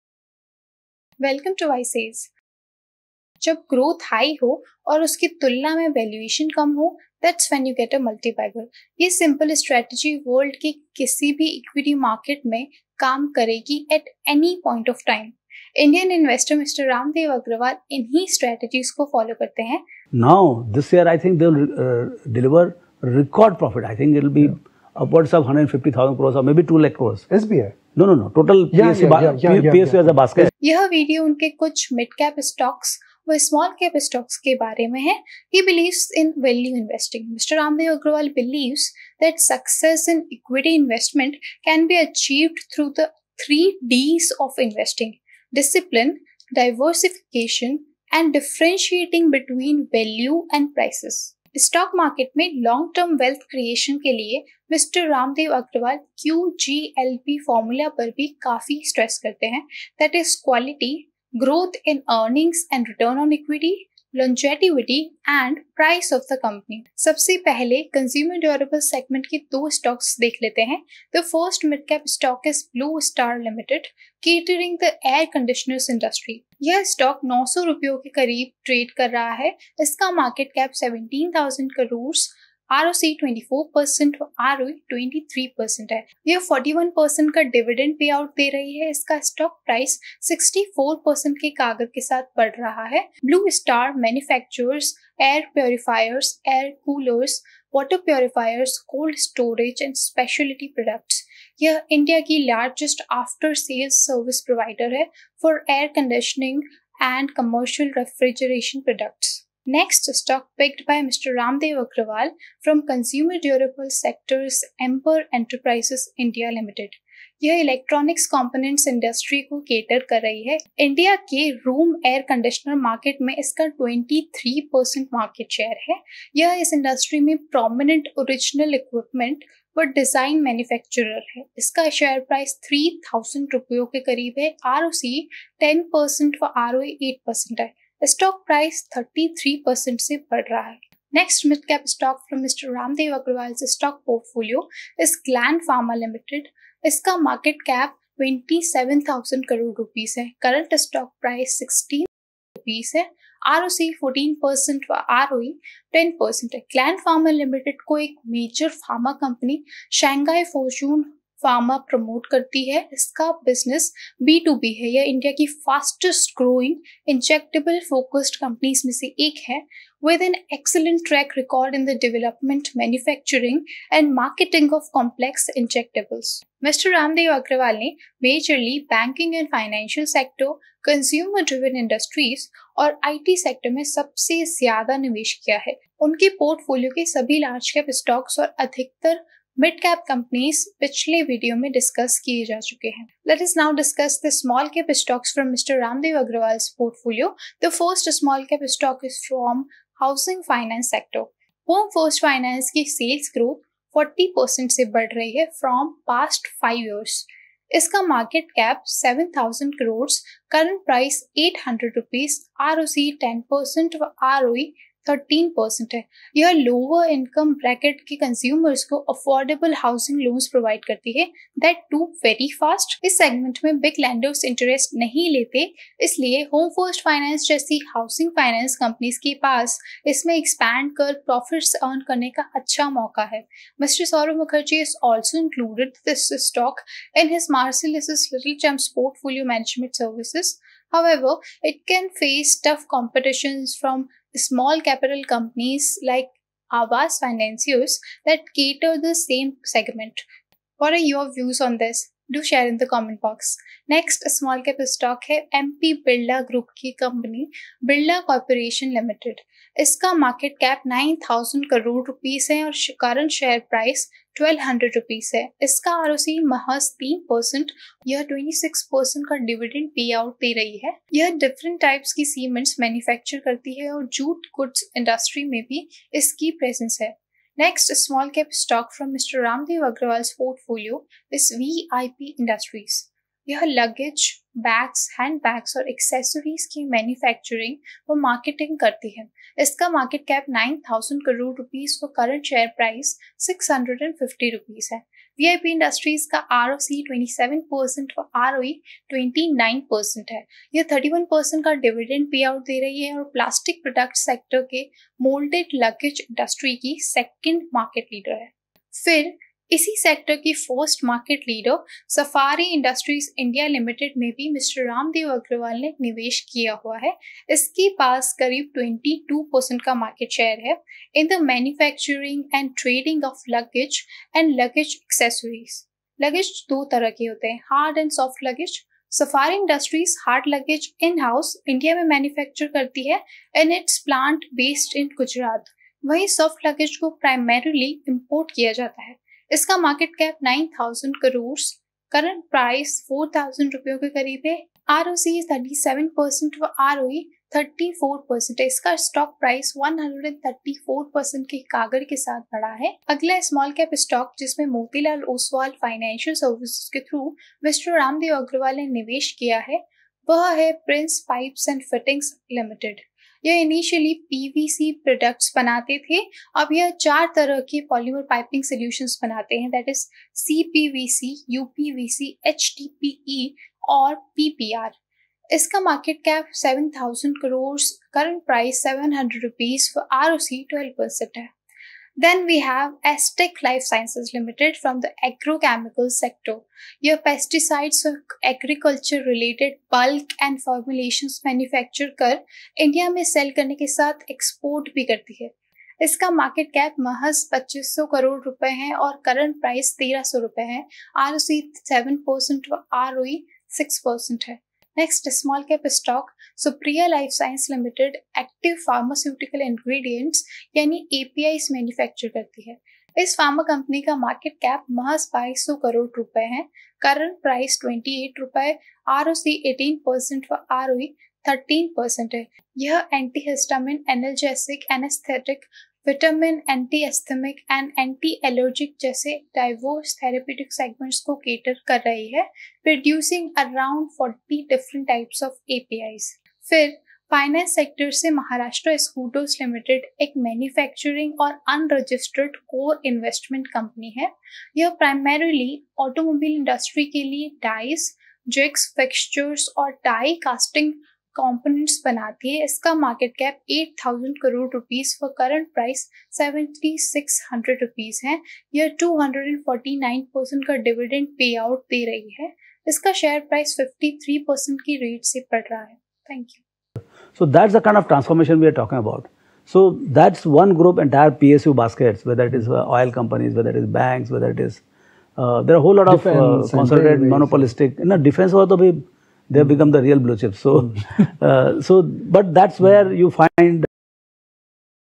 Raamdeo Agrawal Small Cap Stocks Revealed! Multibagger Portfolio 2023
Ramdeo Agrawal Small and mid cap Portfolio Stocks 2023 Revealed ! What is Indian Investor and Chairman of Motilal Oswal Financial Services Mr Raamdeo Agrawal bullish on? His investment style and major sectors. His mid cap and small cap portfolio has been revealed in this video.